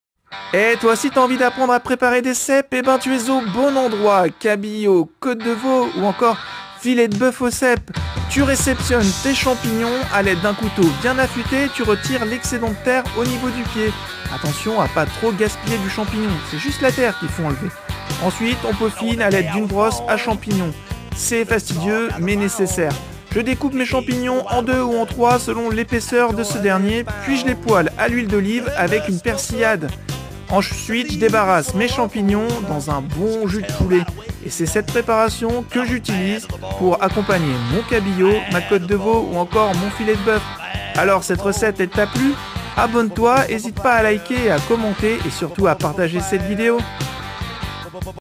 Je Cuisine Des Cèpes 🤤
Aujourd'hui, je vous emmène dans l'univers délicieux des cèpes Découvrez comment préparer un succulent dos de cabillaud aux cèpes et une tendre côte de veau aux cèpes...
Ces plats sont parfaits pour profiter de la saison des champignons.

Amicalement

Pierre 👨‍🍳